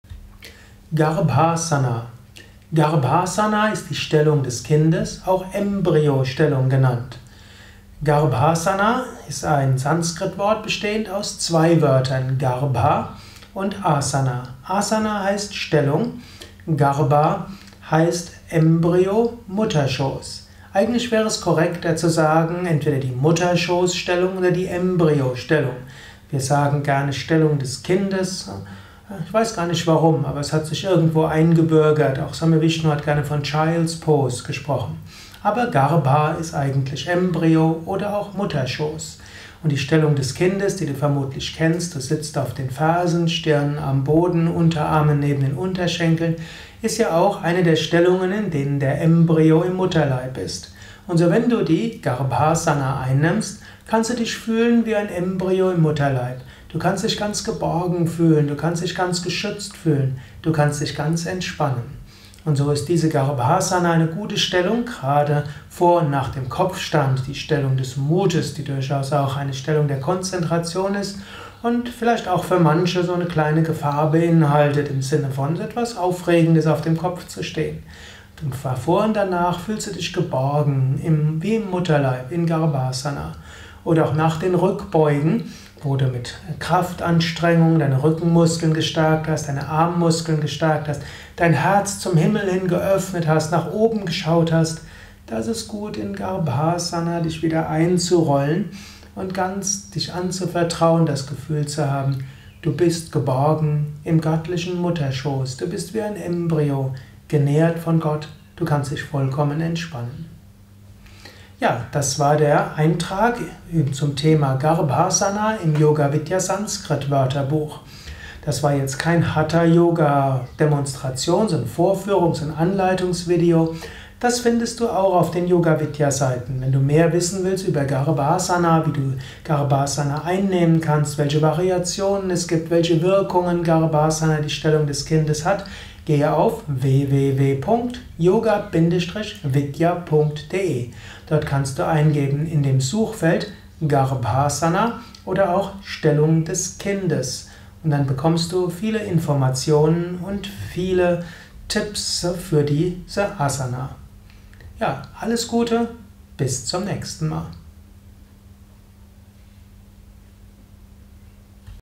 Garbhasana - Stellung des Kindes - Hatha Yoga Wörterbuch
Garbhasana ist die Stellung des Kindes im Hatha Yoga. Sie wird auch als Embryo bezeichnet. Asana heißt Stellung, Haltung. Garbha heißt Bauchhöhle und Embryo. In der Embryostellung kann man sich geborgen fühlen wie im Mutterleib. Letztlich sind wir alle geborgen im Kosmischen Mutterleib.
du einen umfangreichen Artikel zu Garbhasana, mit vielen Erläuterungen, weiterführenden Links, das Wort in der Devanagari Schrift und in der IAST wissenschaftlichen Transkription mit diakritischen Zeichen. Dieser Video Vortrag von und mit Sukadev Bretz zum Sanskritwort Garbhasana ist Teil des Yoga Vidya Multimedia Sanskrit Wörterbuchs